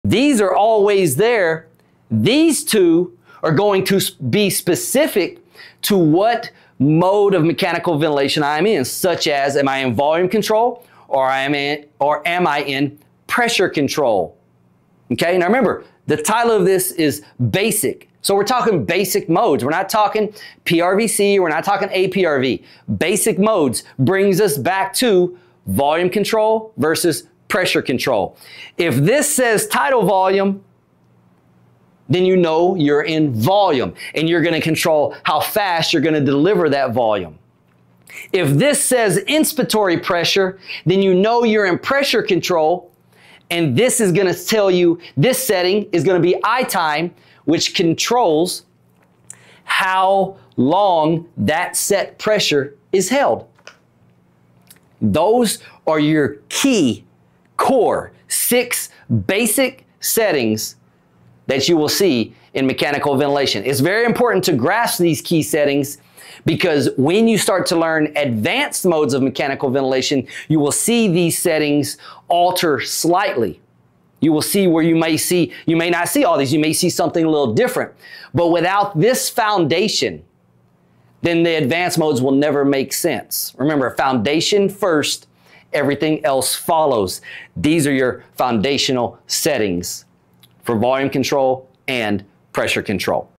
Respiratory Therapy - Basic Mechanical Ventilation Settings
These six vent settings are the foundation of mechanical ventilation. If you understand what each one controls, you’ll understand why your patient responds the way they do. Master the basics and everything else gets easier.

Full YouTube video: Respiratory Therapy – Basic Settings for Mechanical Ventilation